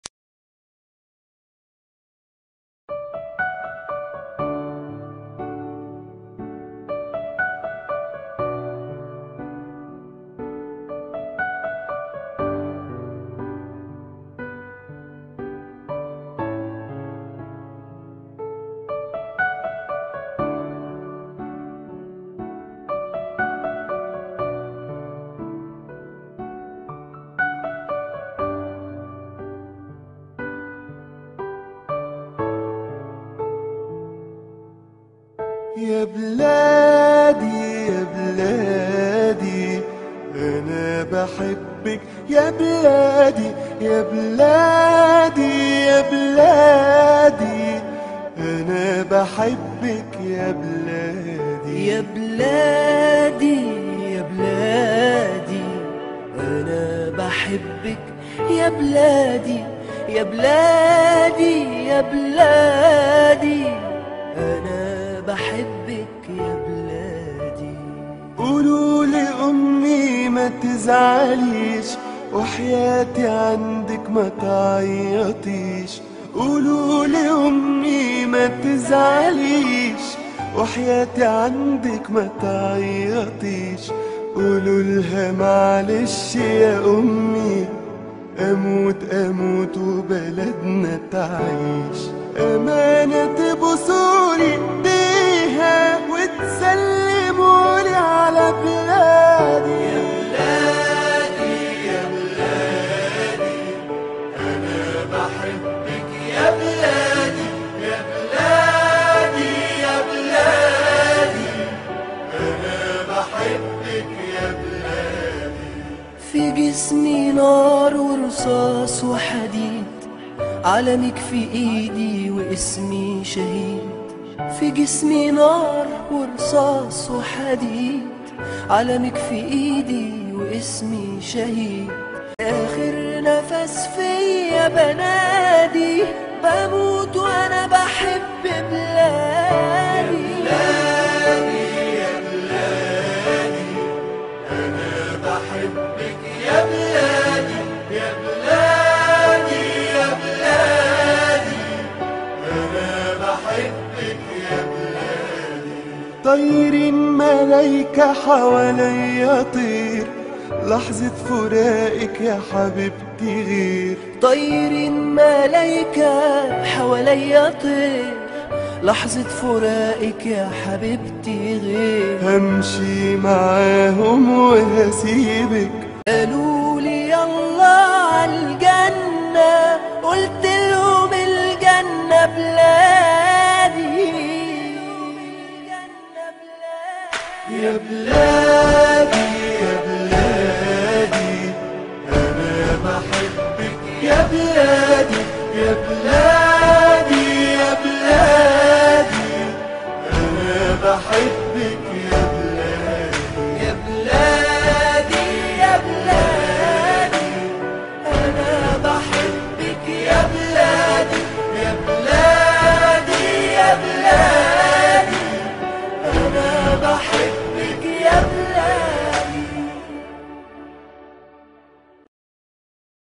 The Martyr Aref El-Mansouri ~ الشهيد عارف المنصوري
Aref El-Mansouri died for the freedom of others in on  2/21/2011

توفي عارف المنصوري عن لحرية الآخرين في يوم 2011/02/21